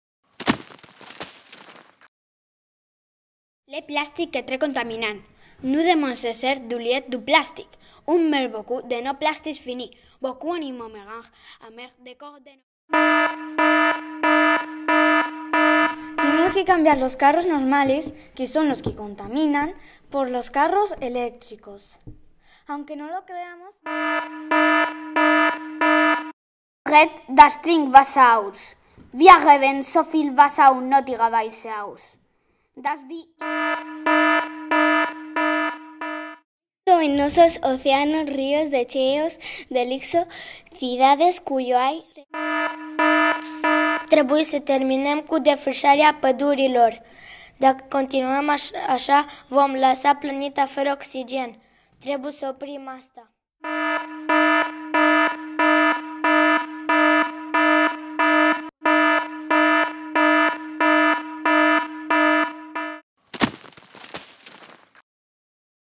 Blabladurías
Todos tenemos mucho peso en el cambio de conciencia medioambiental...Pero quizá muchos de nuestros gobernantes se olvidan que el cambio de legislación para poder llegar a un desarrollo sostenible real y efectivo depende de ellos...Quizá sin tanta blabladuría sería posible.

Compañía de María-La Enseñanza 3º Educación Primaria
Festival de Clipmetrajes de Manos Unidas